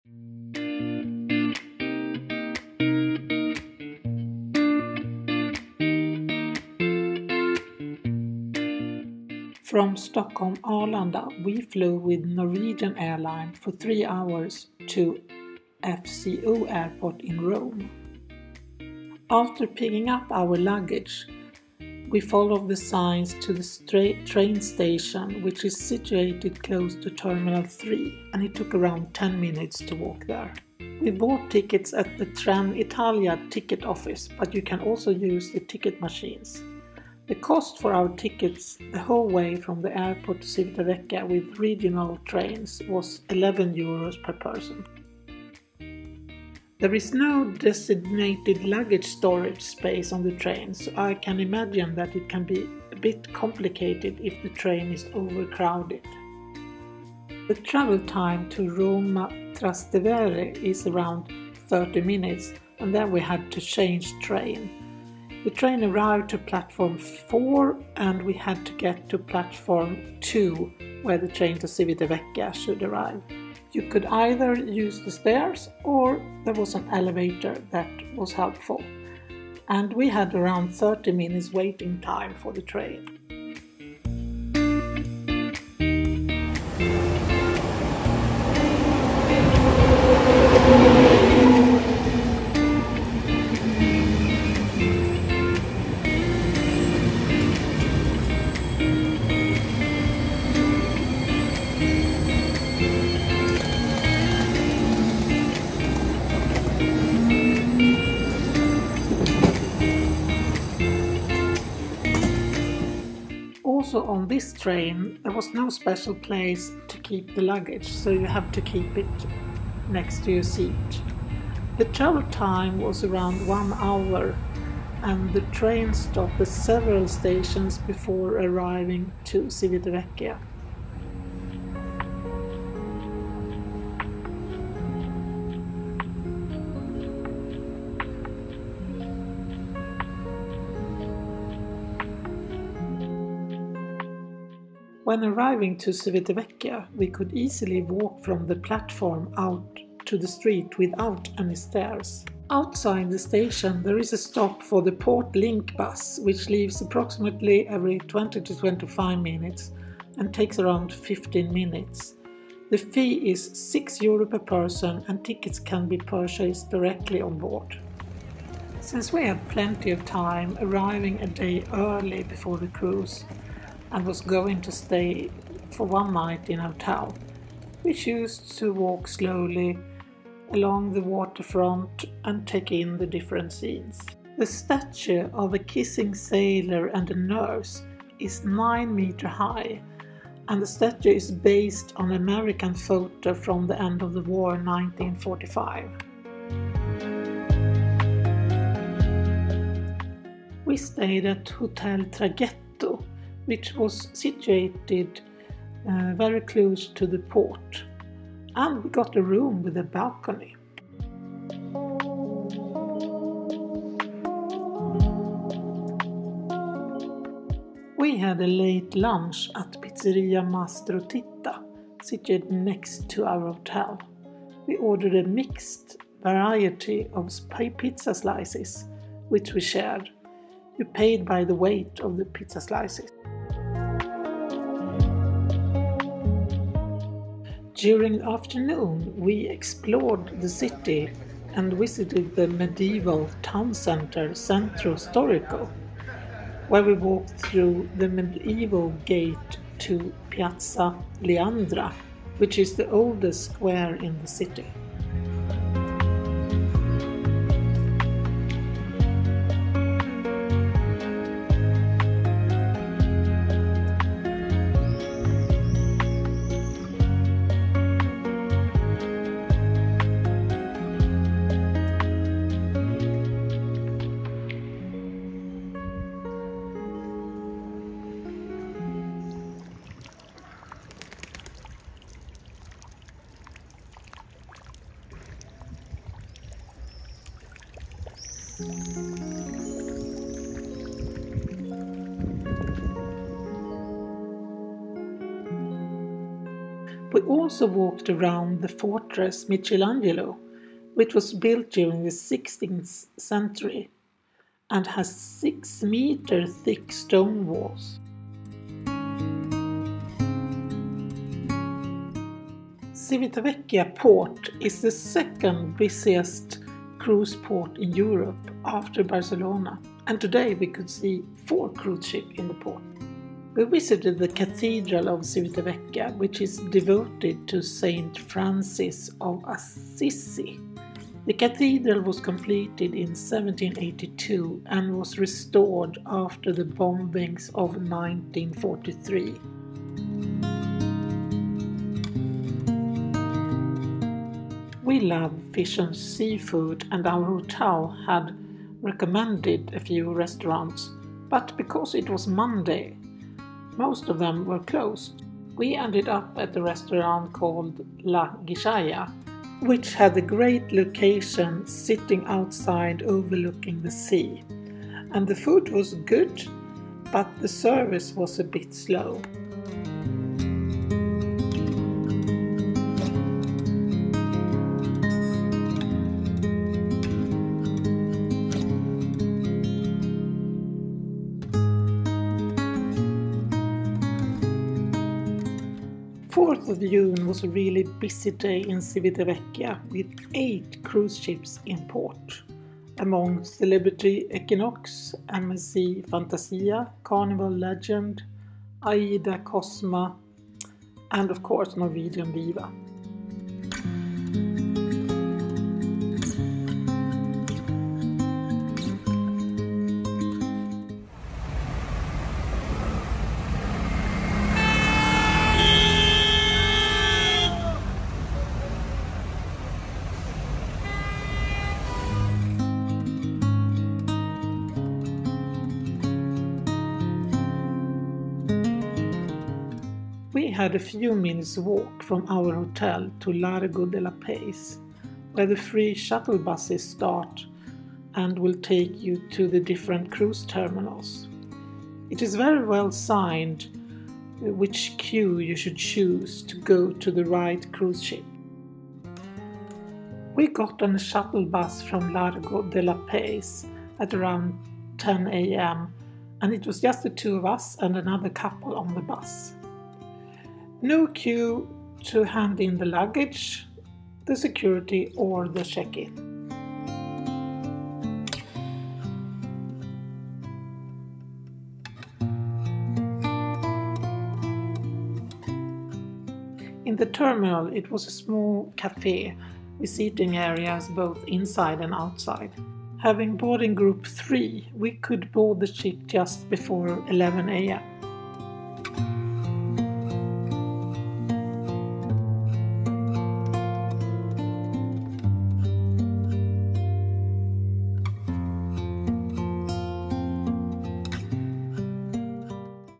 Precruise Civitavecchia 3 and 4 June 2024
Transport from airport FCO Rome to Civitavecchia with regional train. One day in Civitavecchia. Shuttle buses to port in Civitavecchia.

I used music from Tunetank.